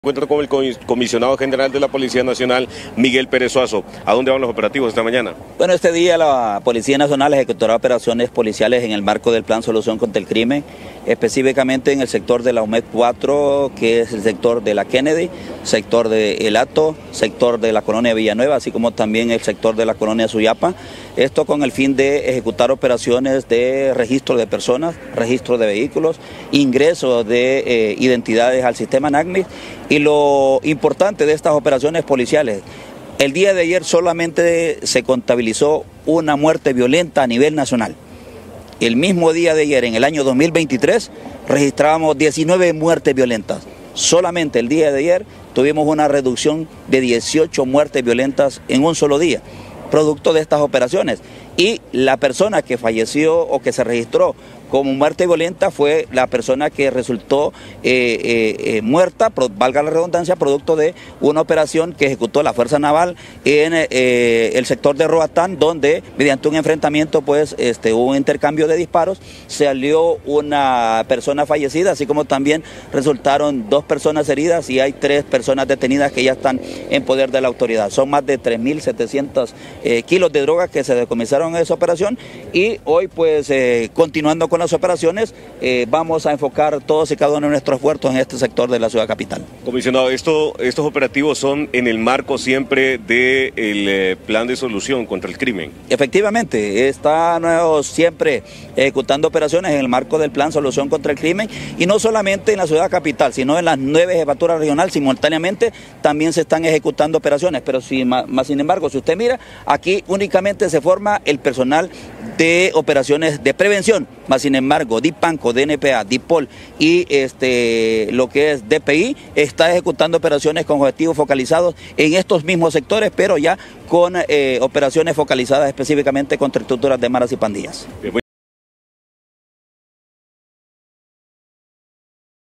Policía Nacional realiza varios operativos en zonas conflictivas de la capital
ICN por la mañana 🌤️ no te olvides sintonizarnos a las 6:00 am de lunes a viernes
🔹TIGO: Canal 33
🔹CLARO: Canal 45
🔹CABLE COLOR: Canal 95
🔹MAYAVISIÓN : Canal 2-99